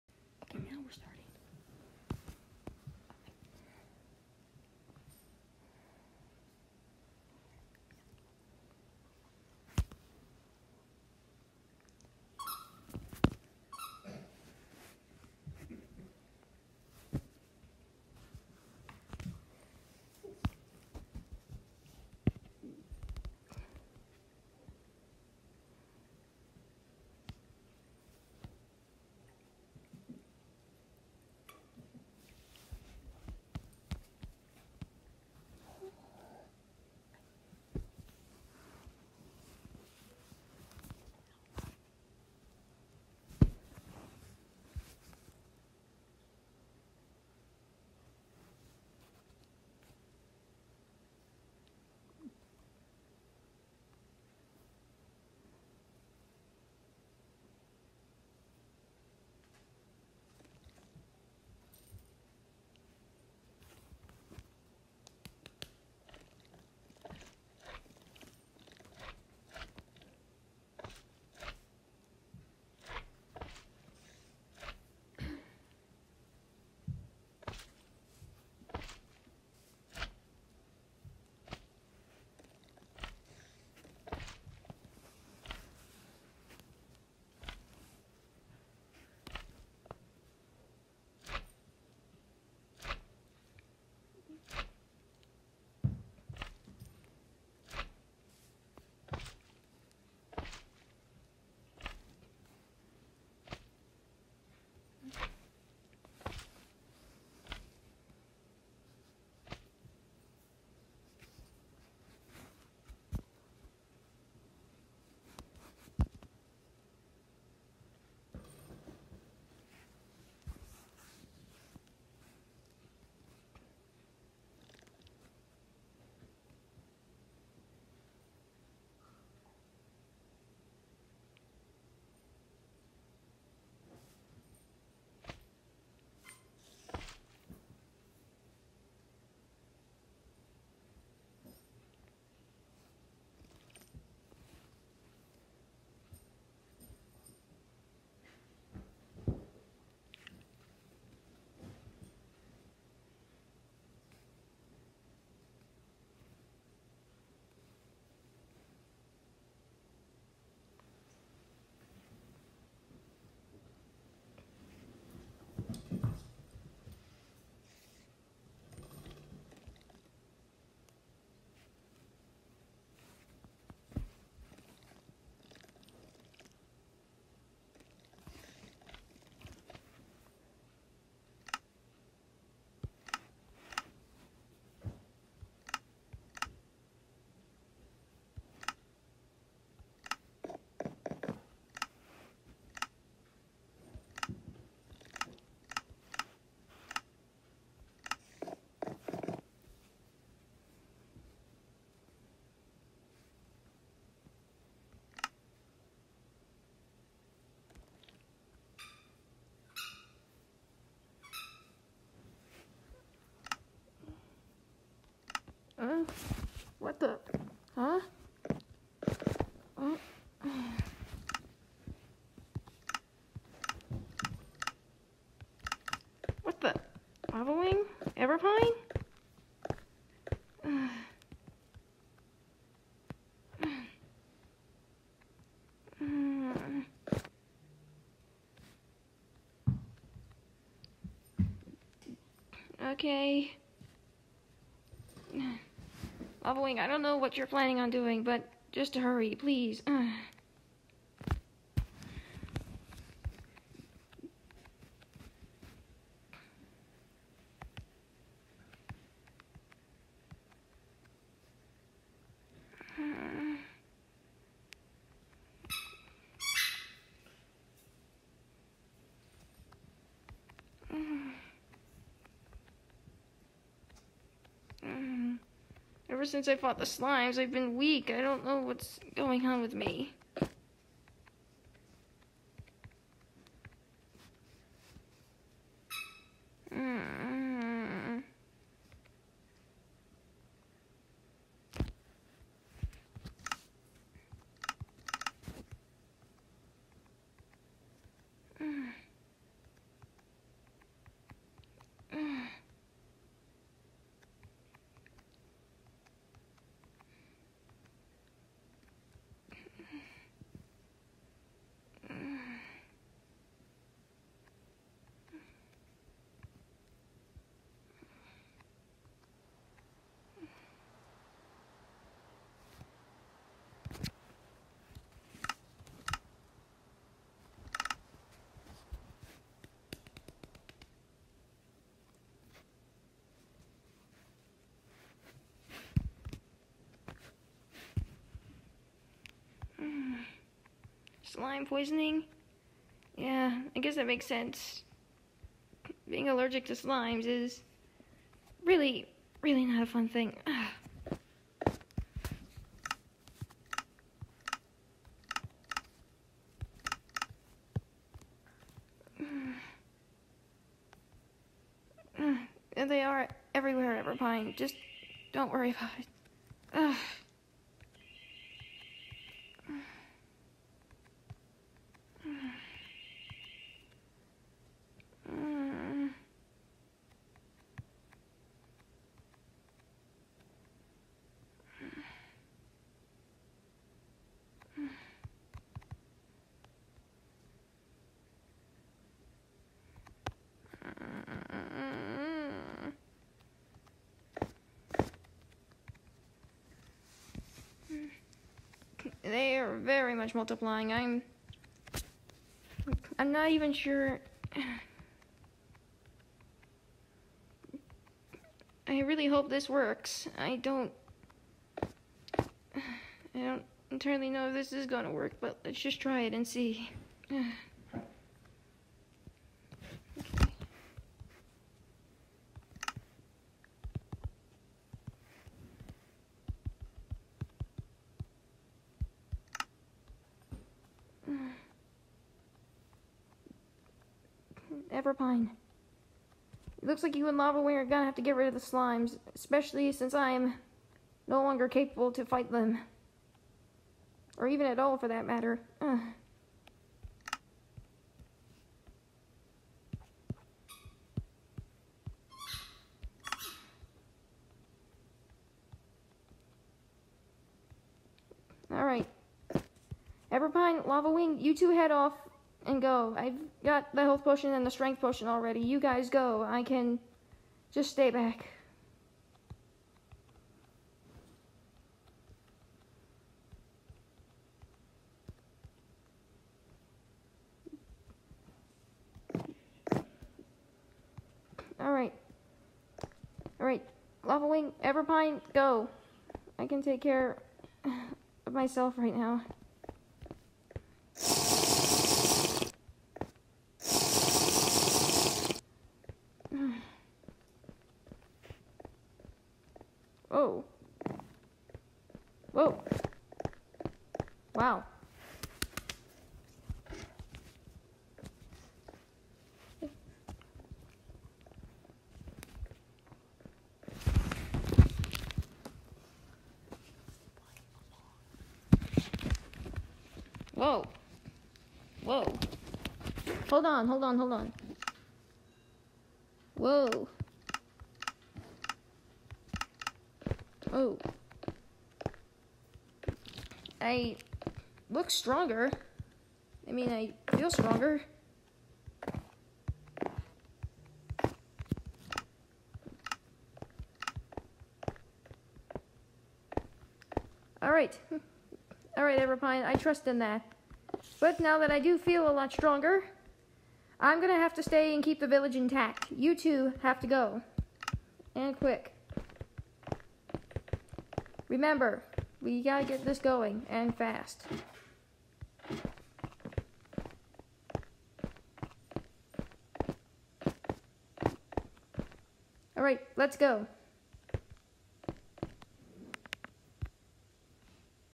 Legend Of The Dinosaur Queen Part:14 Lavawing Saves My Life
Everpine Now Very Concerned For Her Queens Recovery Stumbles On A Familiar Face Lavawing The Mutant Form The a Lava Temple He Asks To See His Struggling Queen Only To End Up Saving Her Life.